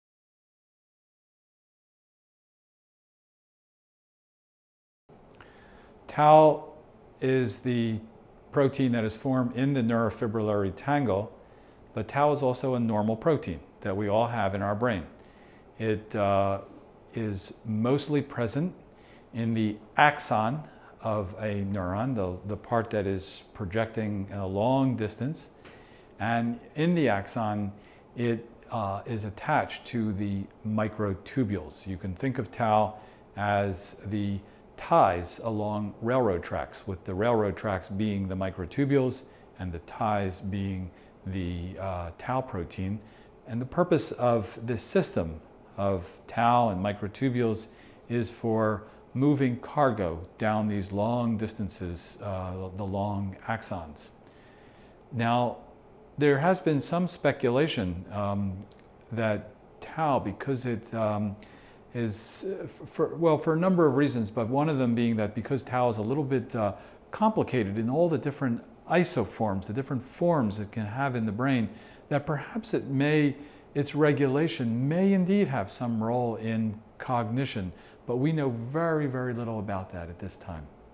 Alzheimer's Disease - Tau Protein (3 of 5)
Professor Kenneth Kosik discusses the tau protein and its relationship to the neurofibrillary tangles found in Alzheimer's disease.